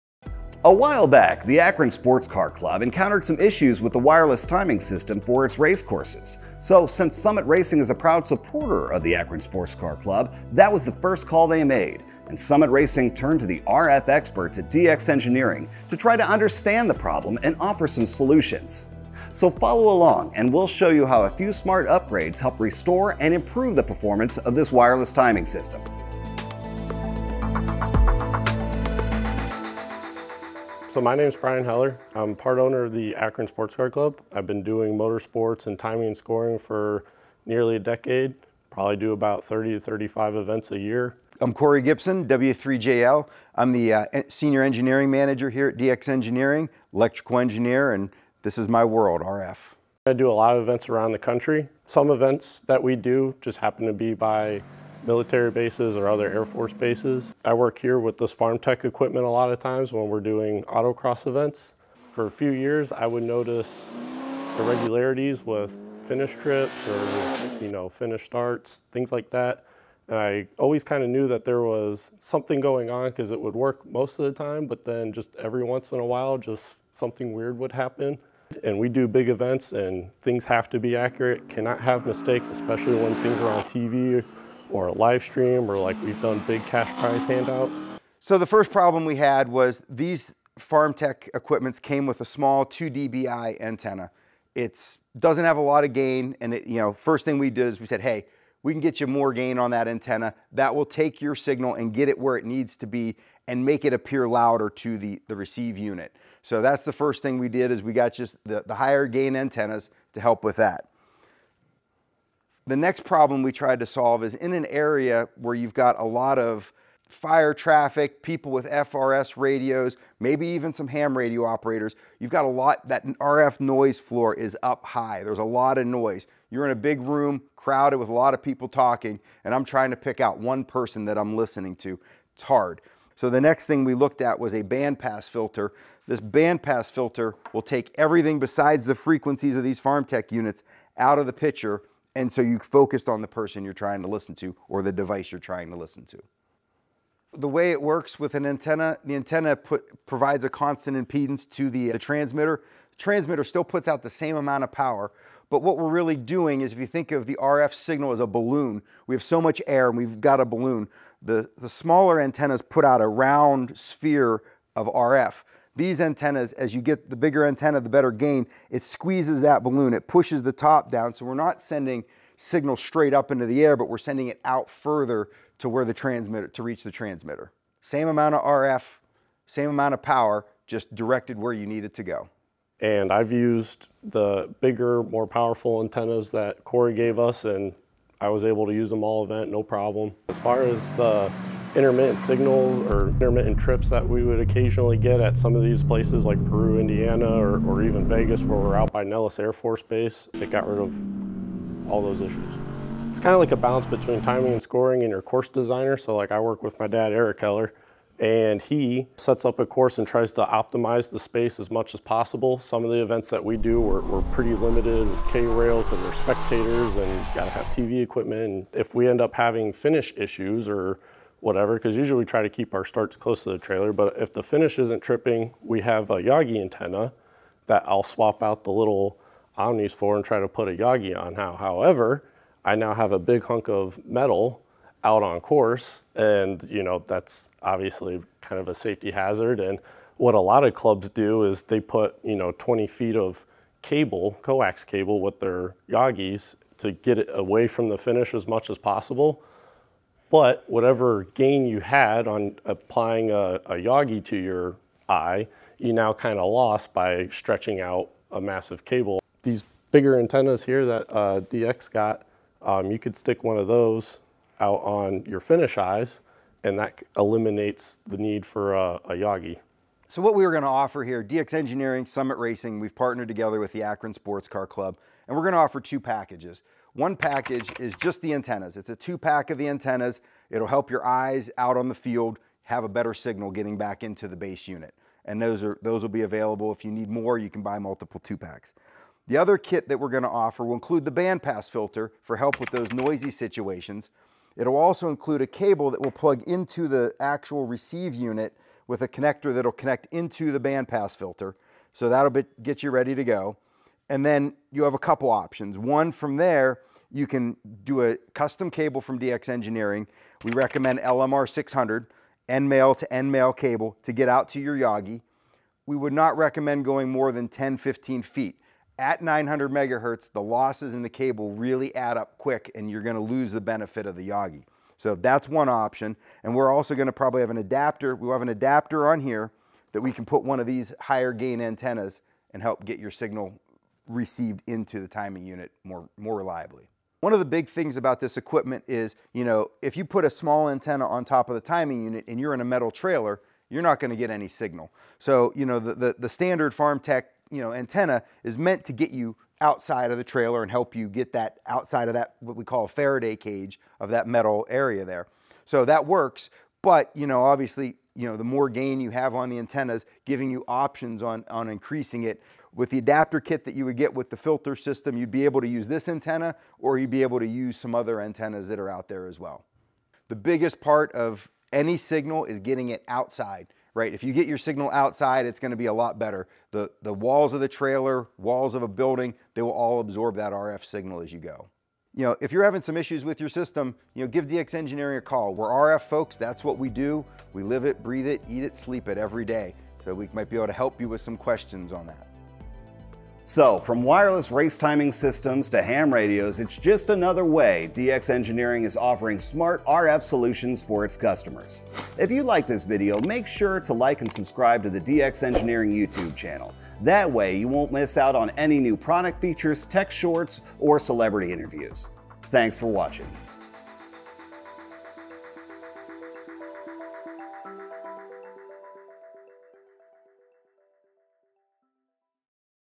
DX Engineering & Summit Racing Offer Improved Track Timing Antennas & Filters
When the Akron Sports Car Club was having problems with its wireless FarmTek track timing system, its folks called Summit Racing—and Summit Racing turned to the radio communications experts at DX Engineering to solve the problem. Turns out, the issue was a one-two punch of under-performing antennas coupled with too much electrical “noise” at the track. DX Engineering solved the problem with a set of new, high efficiency antennas to improve the timing transmitter range, and a band-pass RF filter to eliminate all the extra radio noise—and they’re offering handy kits so you can upgrade your timing system the same way. If you’re having similar issues with your FarmTek wireless timing system for your racetrack or autocross club, this video is worth a watch.